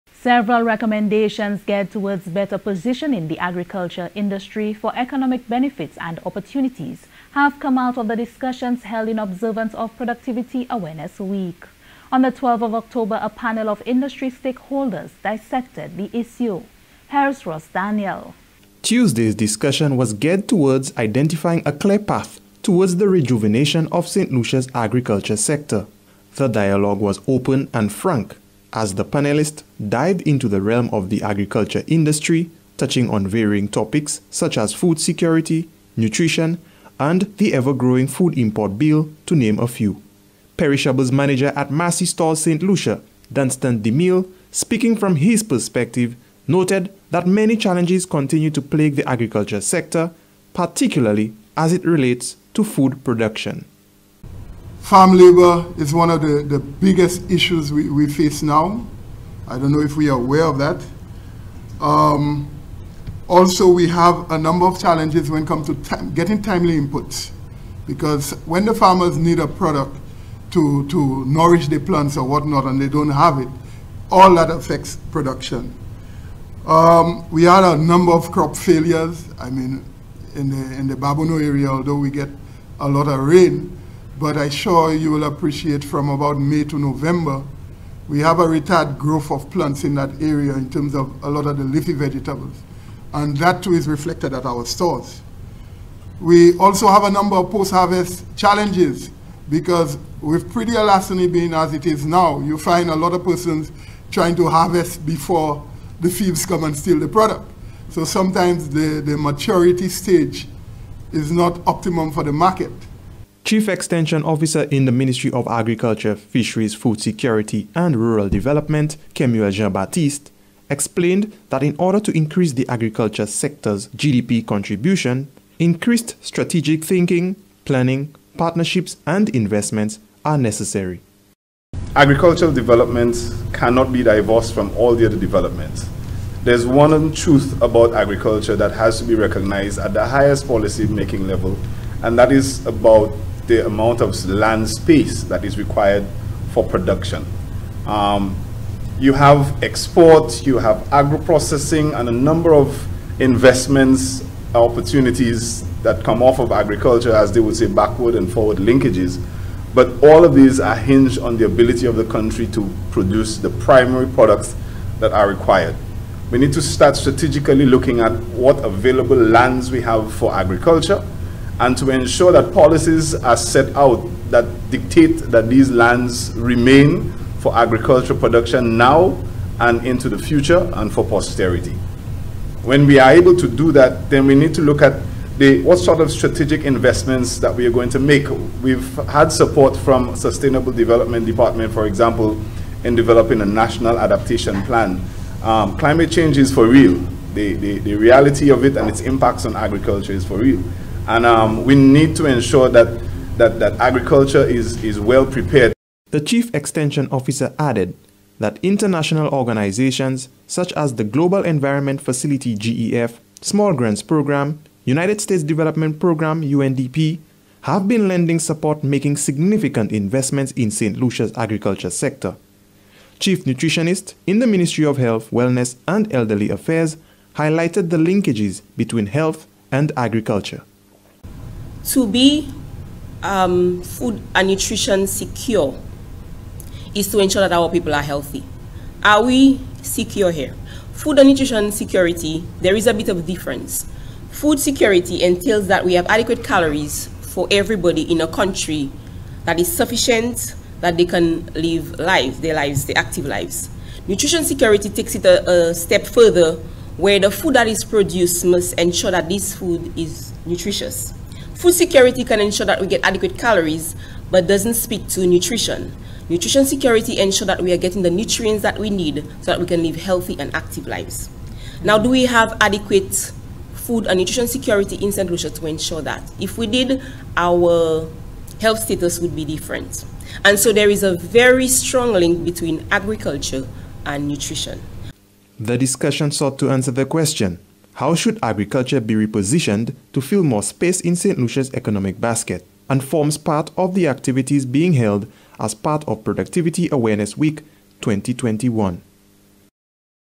Agriculture sector a priority
Several recommendations geared toward better positioning the Agriculture sector for economic benefits and opportunities have come out of discussions held in observance of Productivity Awareness Week.